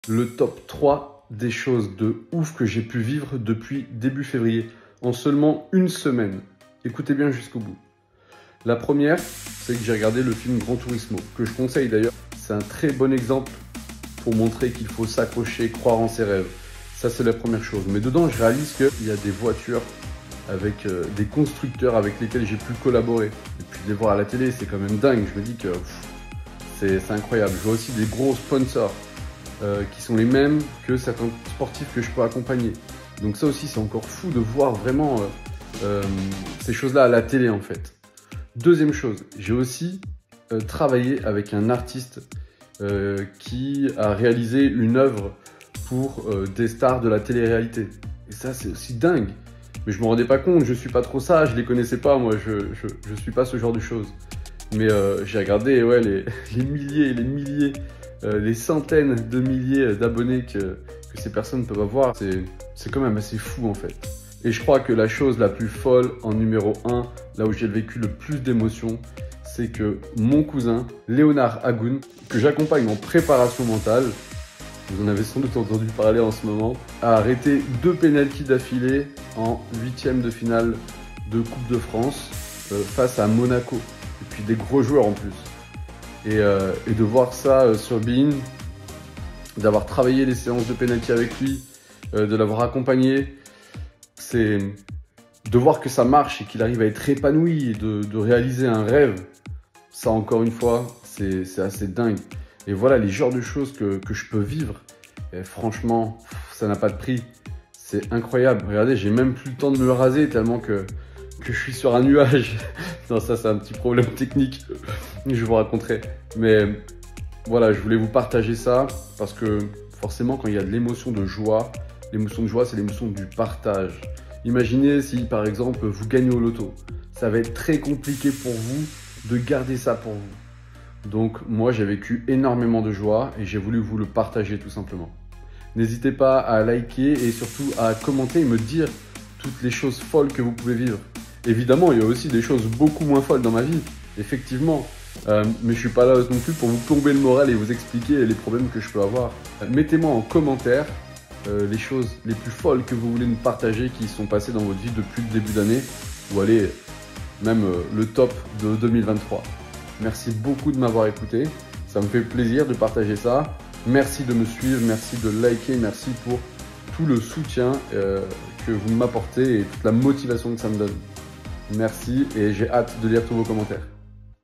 📺 TV réalité, exploits sportifs, entreprises mythiques, … au cœur de la médiatisation !
La médiatisation à un pouvoir particulier, travailler avec des personnes ou des organisations médiatisées est comme une récompense, une validation du travail accompli dans l’ombre, qui se voit reconnu à travers les émotions intenses et la reconnaissance publique.